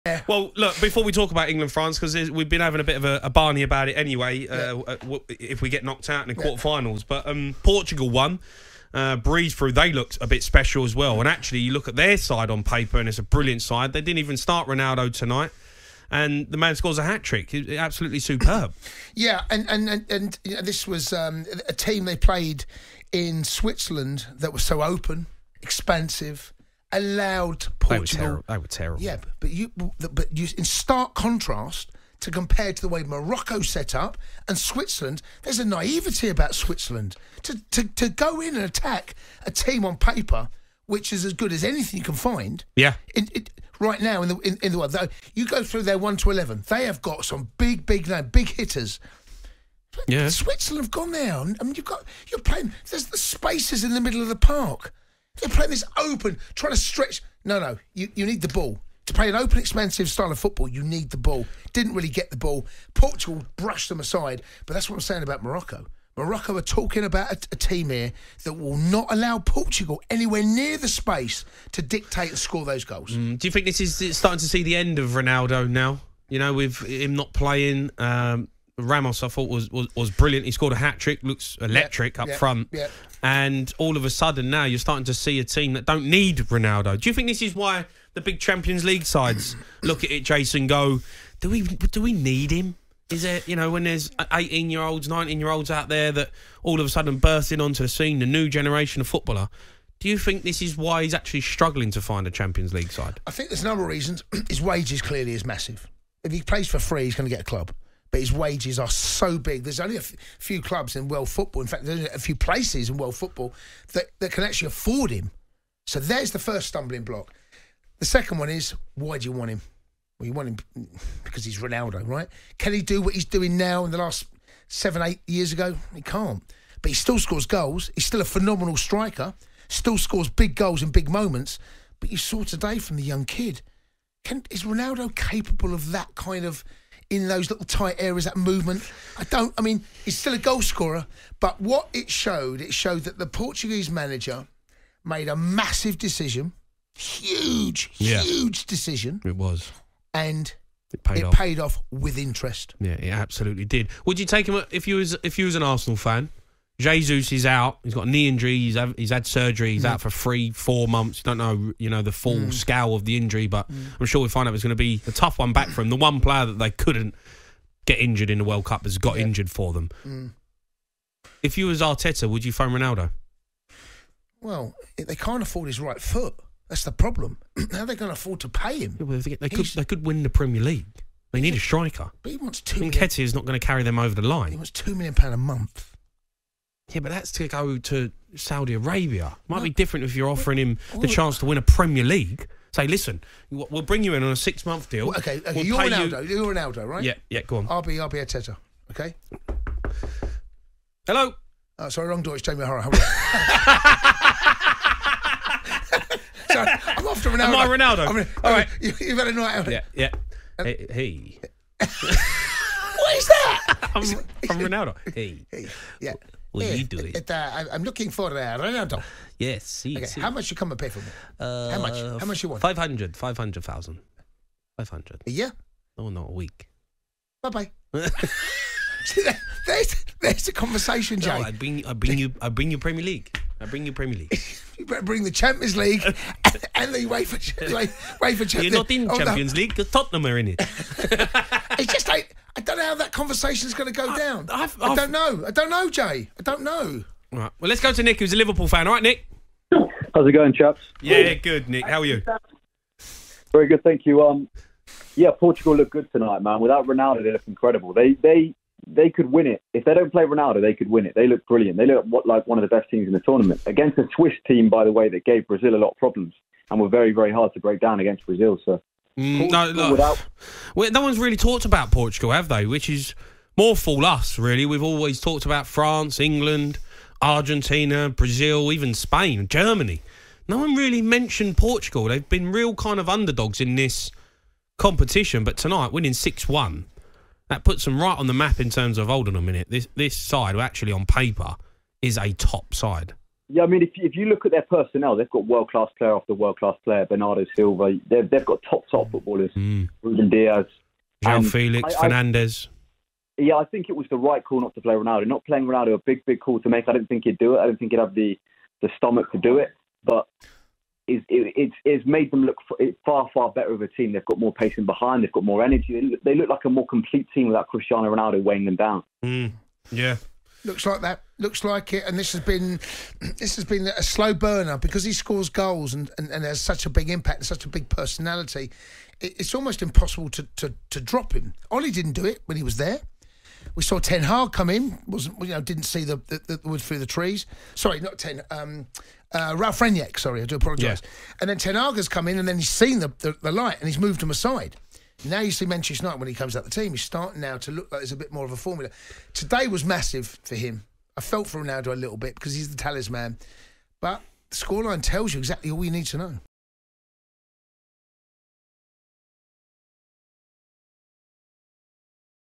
This Liverpool fan says Portugal can win the World Cup IF they don't start Ronaldo! 👀🤔
Cristiano Ronaldo was benched for Goncalo Ramos, who scored a hat-trick, & now many are suggesting Portugal are better without Ronaldo in the team!